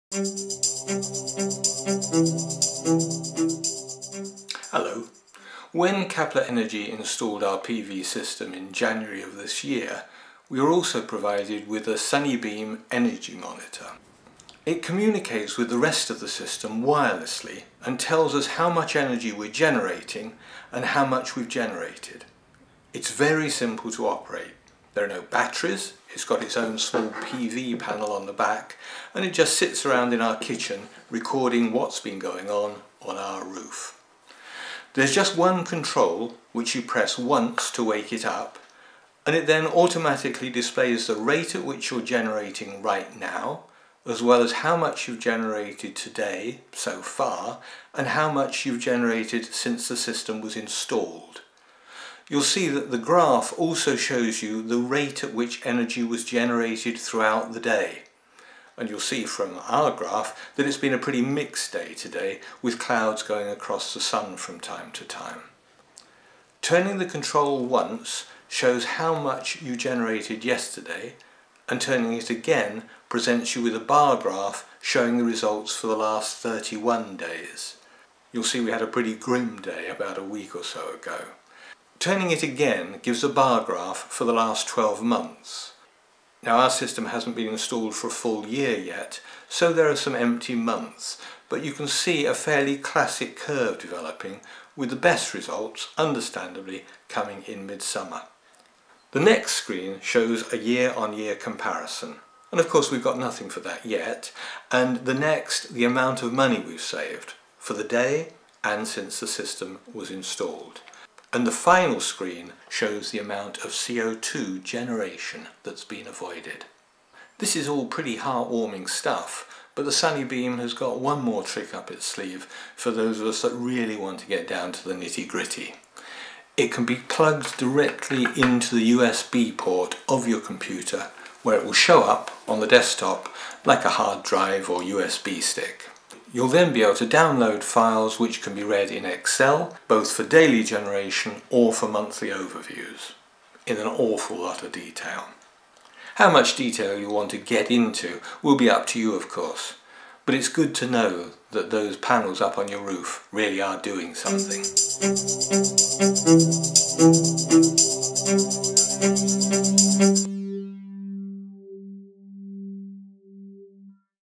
Caplor Sunny Beam
A video from Caplor Energy demonstrating the use of the Sunny Beam Energy Monitor.